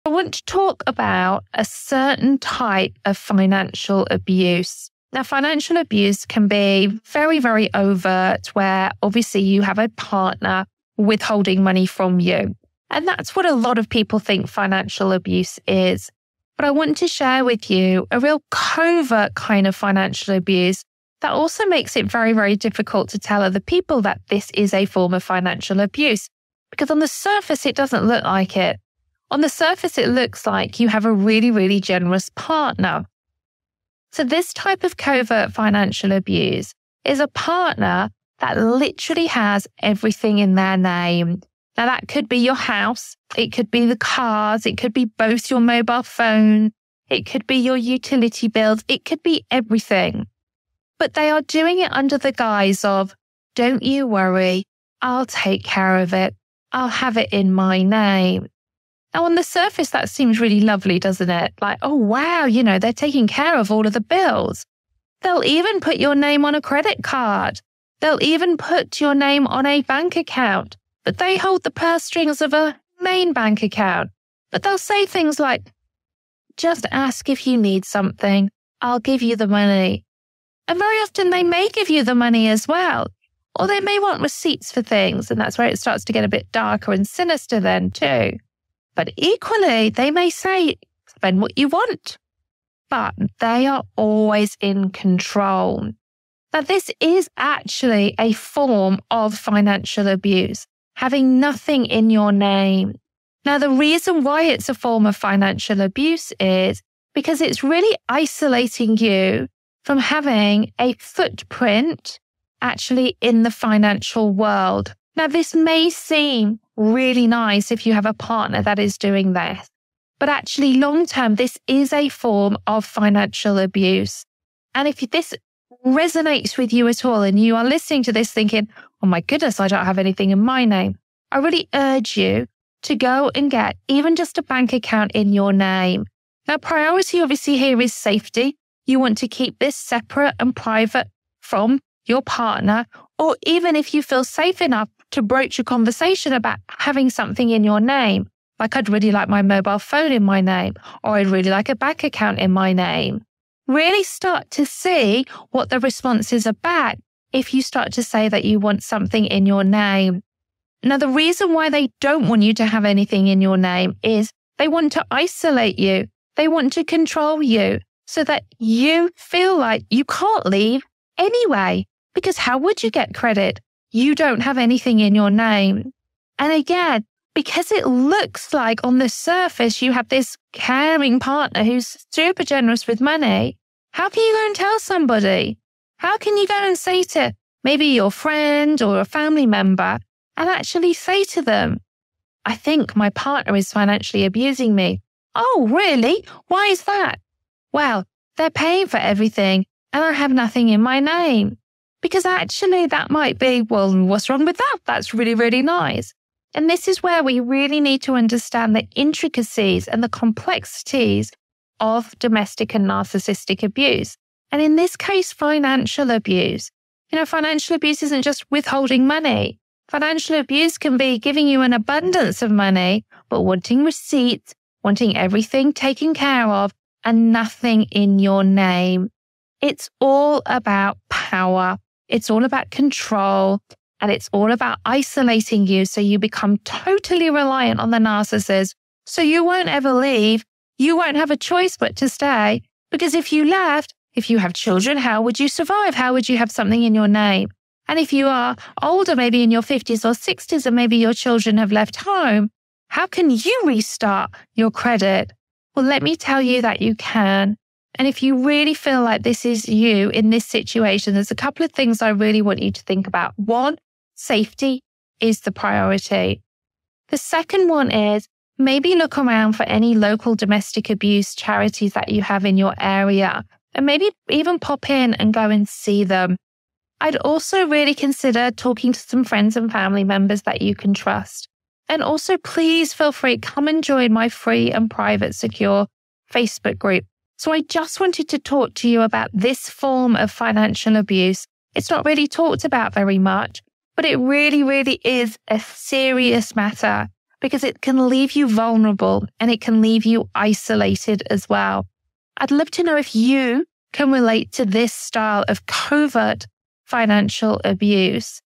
Covert Financial Abuse by the Narcissist
Have you ever heard your partner say, "Don't worry, I'll handle everything and put it in my name," or "Just let me know if you need anything"? While these phrases might seem like generosity, they can actually be signs of financial abuse. In this episode, I’ll explore covert financial abuse and how narcissists use it to gain control, leaving you with little financial independence and making it hard to leave the relationship. But fear not, I'll guide you on reclaiming your financial independence and safely exiting the relationship.

What you will learn in this episode:

How narcissists hide their control behind generosity.
Why so many people suffer from covert financial abuse without noticing.
How to become financially independent and safely leave a narcissist.

I look forward to helping you on your journey to not just survive after narcissistic abuse but THRIVE

Love Caroline Strawson xoxo